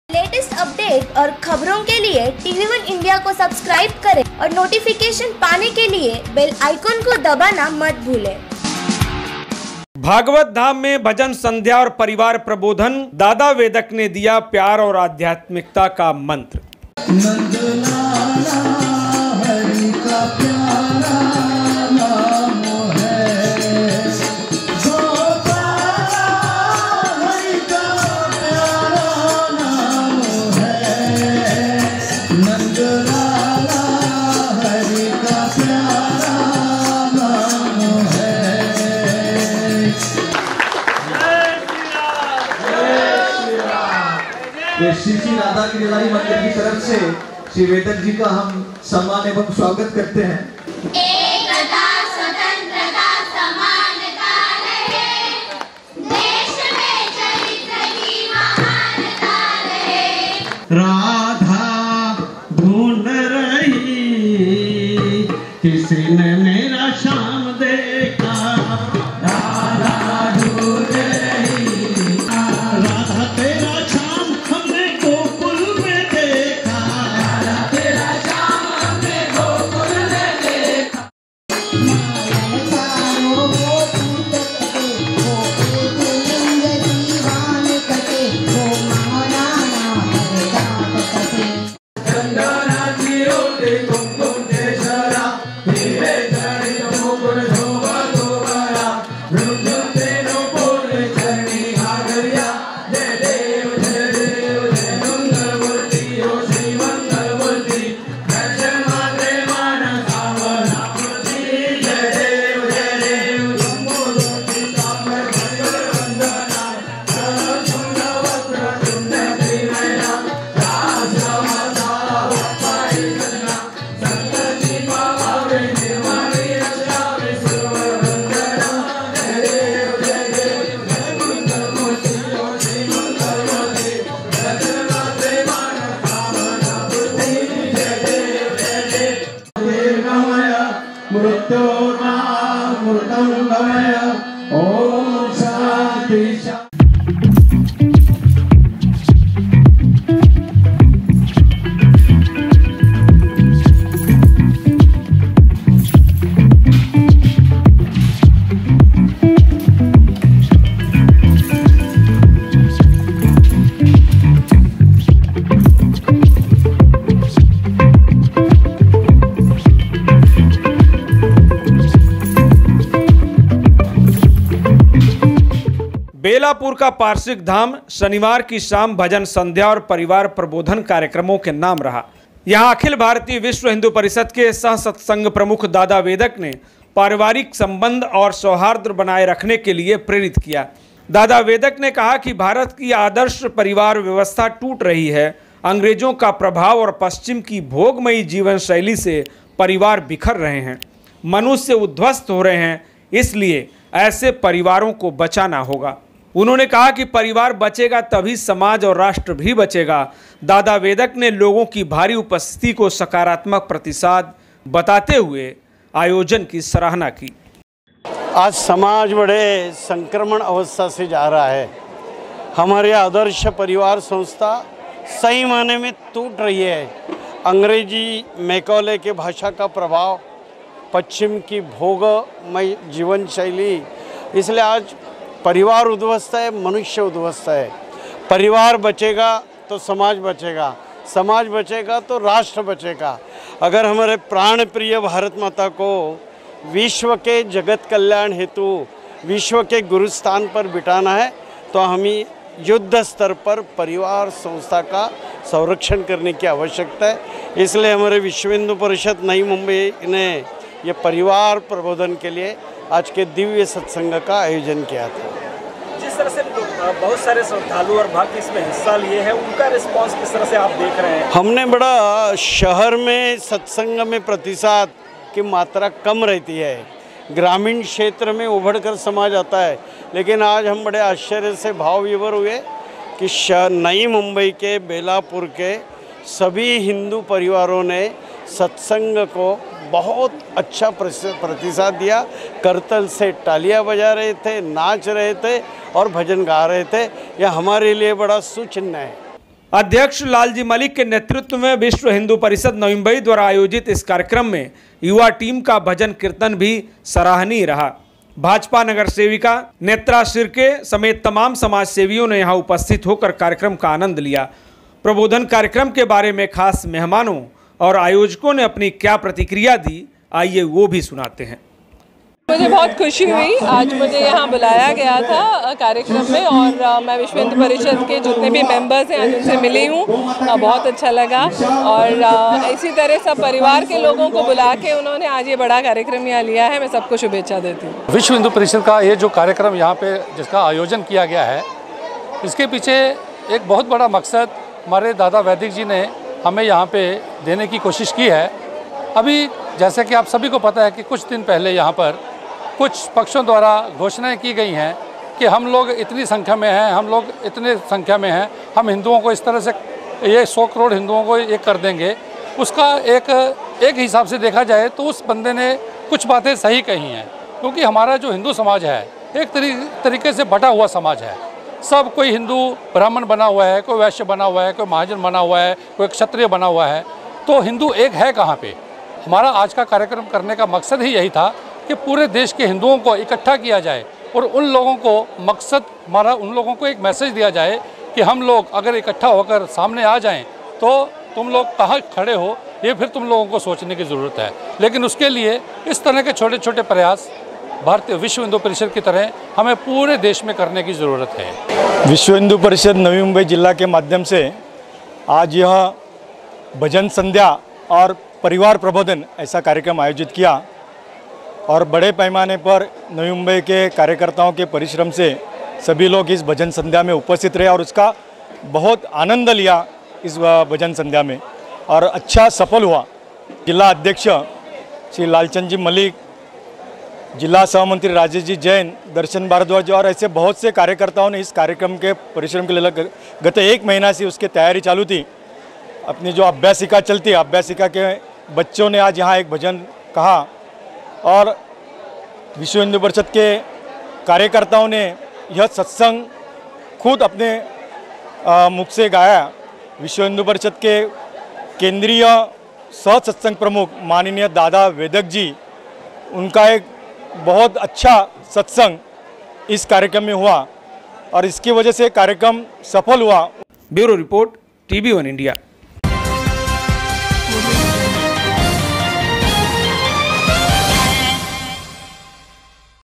Bhagwat Dham में असंख्य परिवारों को प्रबोधन | Dada Vedak ने सिखाया आदर्श परिवार का मंत्र
भागवत धाम में भजन संध्या और परिवार प्रबोधन | दादा वेदक ने दिया प्यार और आध्यात्मिकता का मंत्र . बेलापुर का भागवत धाम, शनिवार की शाम भजन-संध्या और परिवार प्रबोधन कार्यक्रमों के नाम रहा.यहां सैकड़ों परिवार मौजूद थे, जिन्हें अखिल भारतीय विश्व हिन्दू परिषद के सहसत्संग प्रमुख दादा वेदक ने पारिवारिक संबंध और सौहार्द्र बनाए रखने के लिए प्रेरित किया. दादा वेदक ने कहा, 'भारत की आदर्श परिवार व्यवस्था टूट रही है.